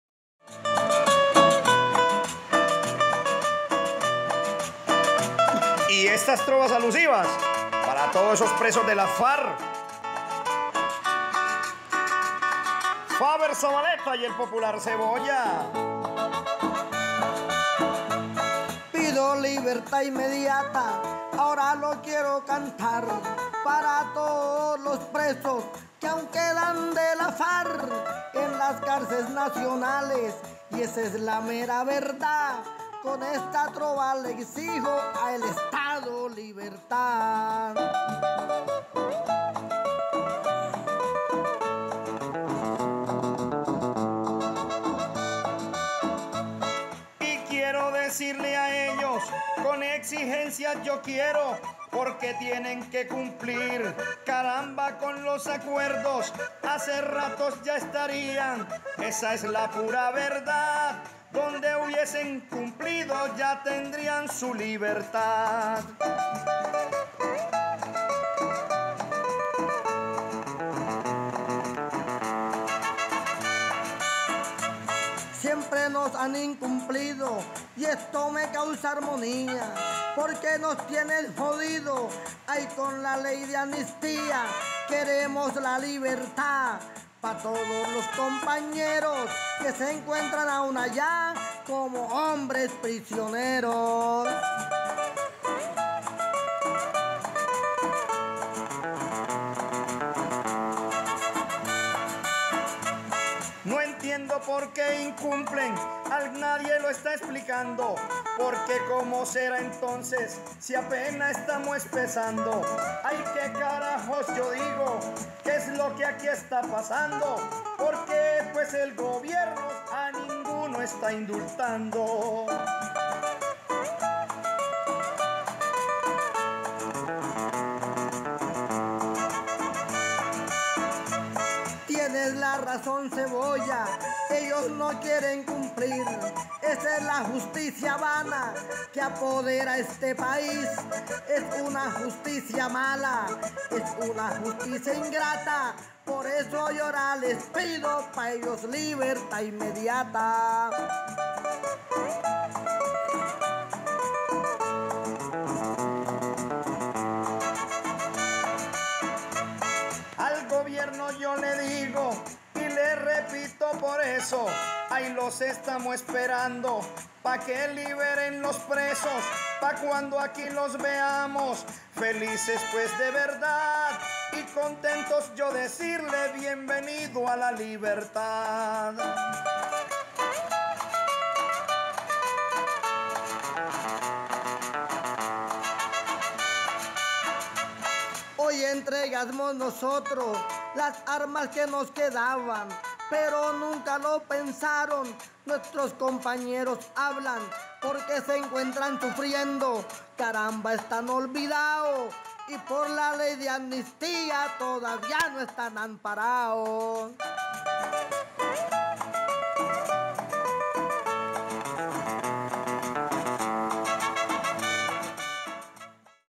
Trovas por la libertad de los prisioneros políticos de las FARC-EP
Trova de los compañeros Faber Zabaleta y Cebolla, ex prisioneros amnistiados, en solidaridad con la Jornada Nacional de Protesta y por la libertad de los prisioneros políticos y la implementación inmediata de la Ley de Amnistía e Indulto.